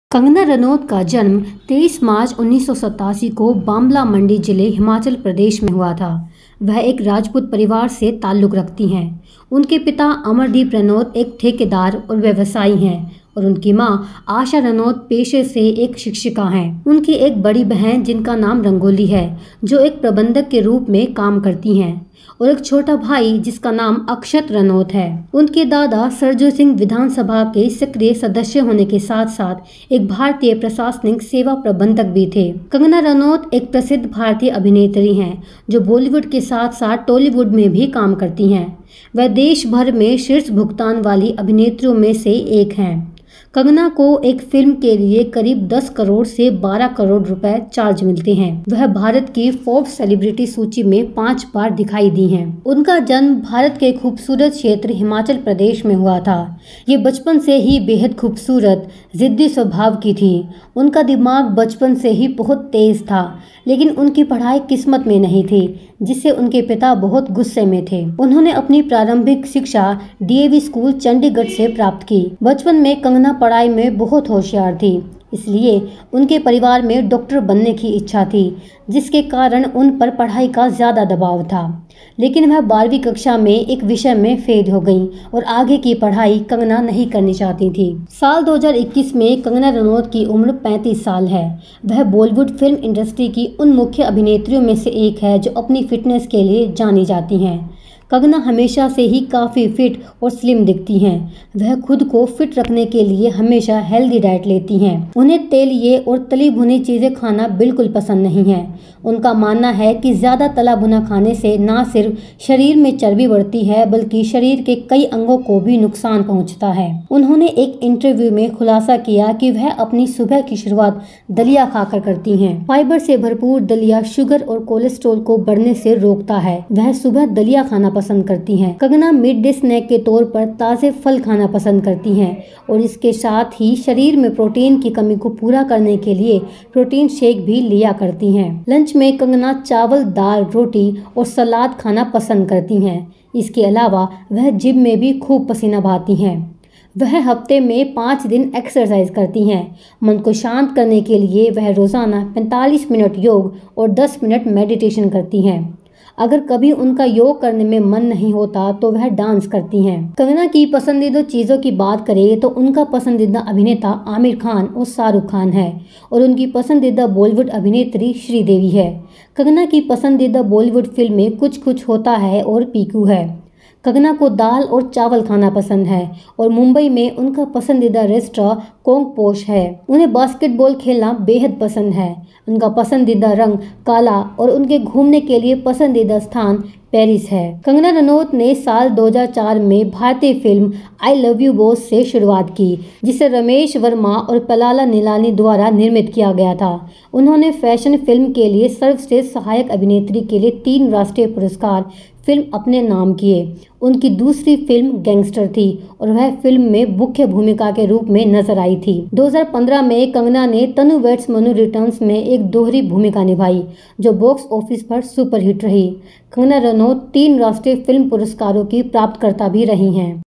Kangana Ranaut Biography Income, House, Boyfriend, Cars, Family, Lifestyle & Net Worth
Kangana Ranaut Lifestyle | Biography & Life Story

Cast & Crew :
Producer/Director : Surender Singh ( Lovely)
Story Wirter :  Sudhir Goutam
Screen Writer / Screen Play : Sajan Baliyan &